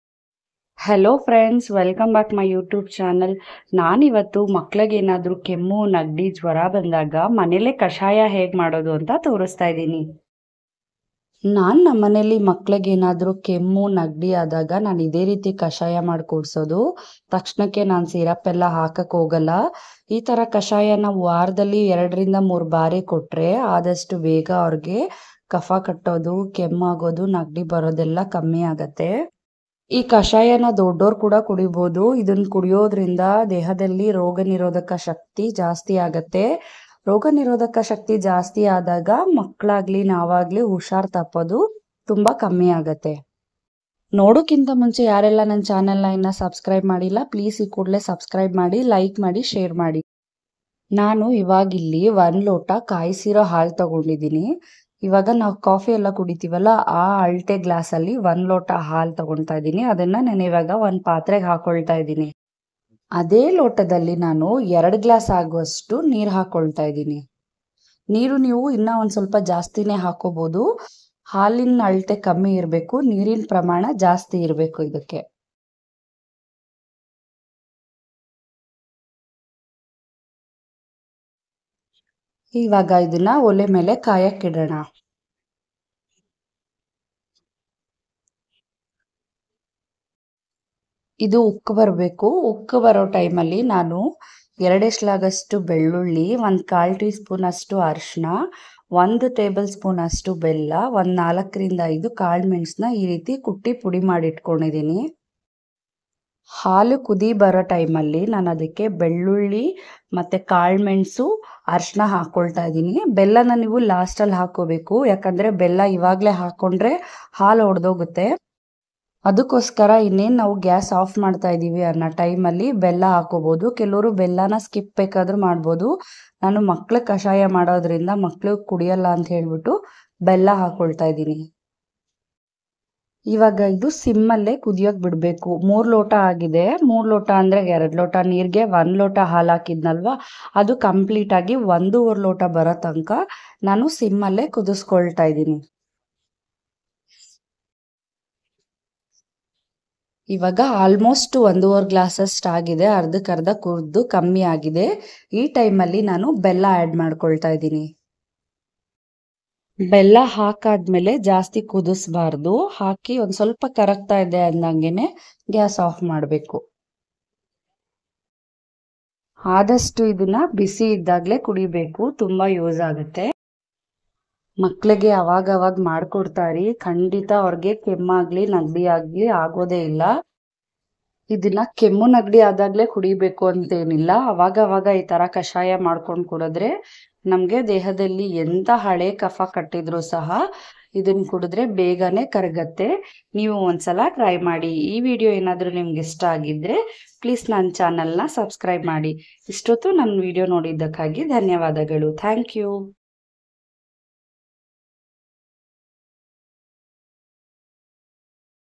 ಕಷಾಯ ಮಾಡುವ ವಿಧಾನ /ಮಕ್ಕಳಿಗೆ ಕೆಮ್ಮು ನಗಡಿ ಜ್ವರ ಬಂದಾಗ ಈ ರೀತಿ ಒಮ್ಮೆ ಕಷಾಯ ಮಾಡಿ nanduprapancha 2024 04 05
ಕಷಾಯ ಮಾಡುವ ವಿಧಾನ /ಮಕ್ಕಳಿಗೆ ಕೆಮ್ಮು ನಗಡಿ ಜ್ವರ ಬಂದಾಗ ಈ ರೀತಿ ಒಮ್ಮೆ ಕಷಾಯ ಮಾಡಿ how to make kashaya for kids